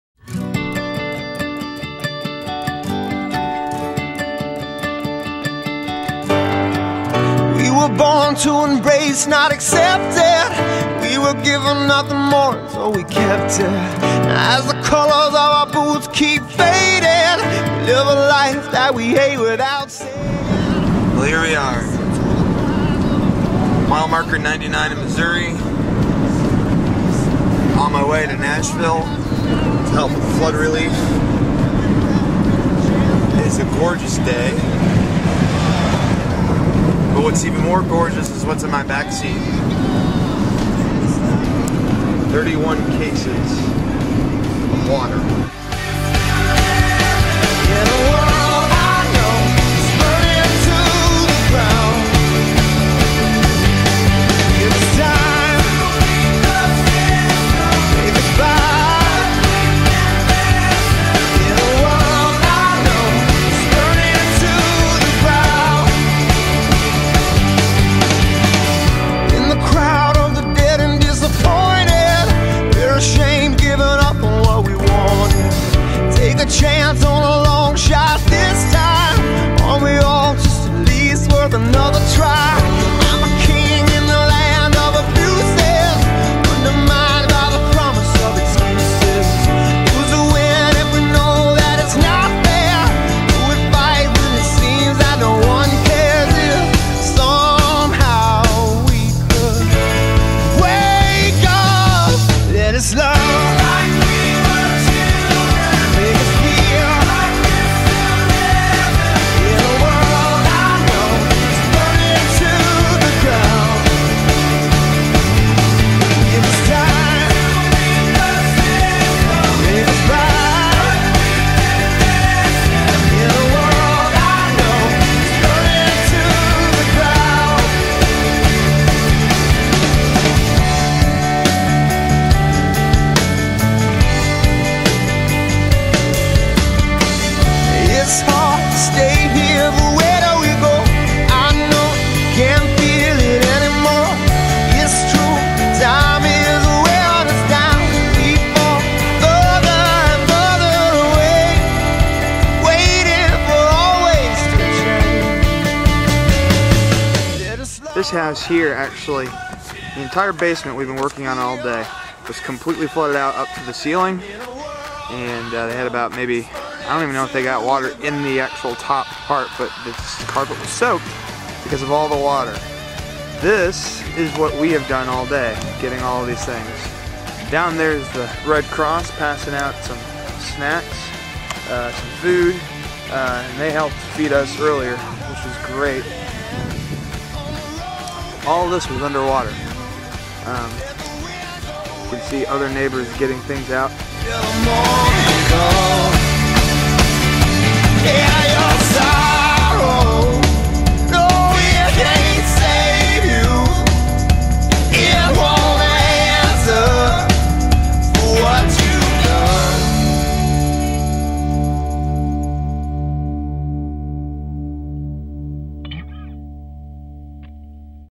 Nashville Clean Up
My small amount of footage from the clean up efforts in Nashville, TN after the floods of May 2010.